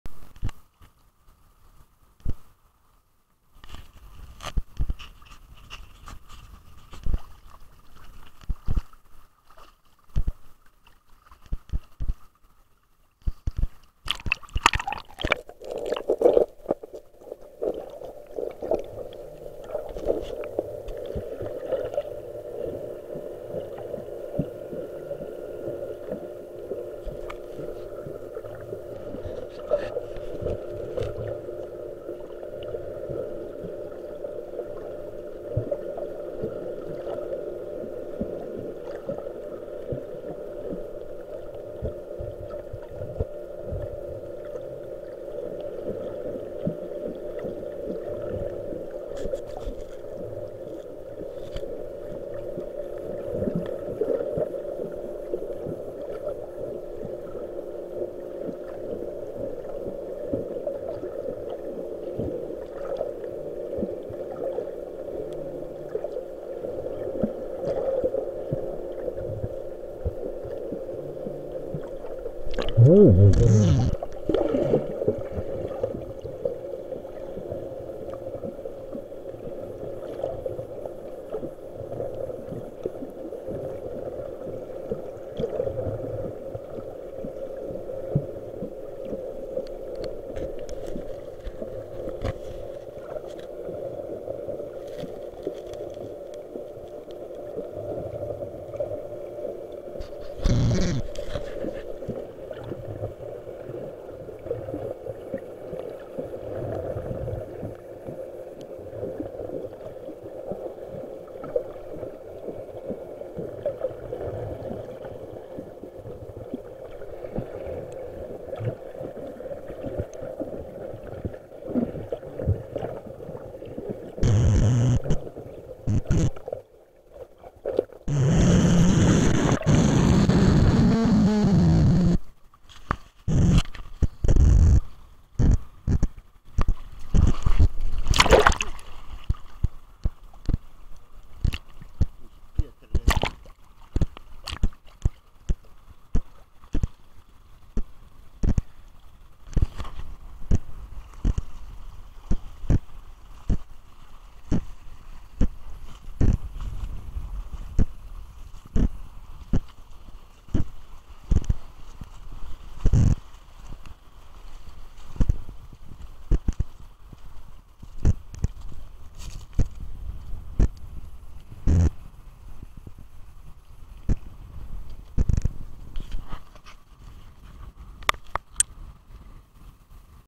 La adunat de scoici
Port Agigea, pe vânt puternic, curenți la fel de puternici.